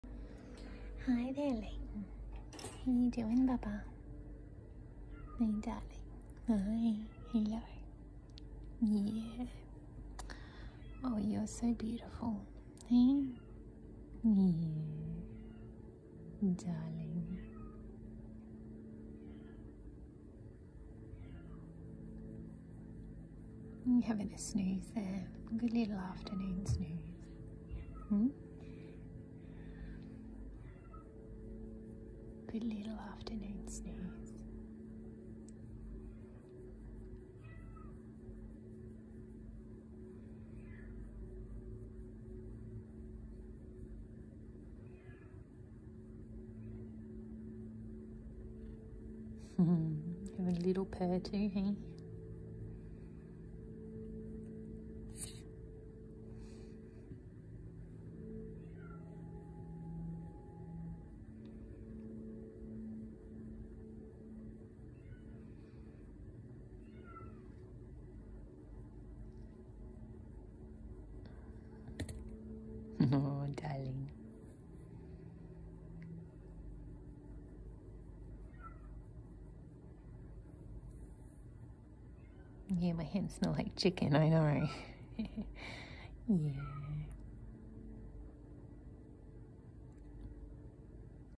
Leyton
I’m a shy but sweet kinda guy. Once I settle in you’ll see how great I am! Adopt me from the Brisbane RSPCA (Wacol)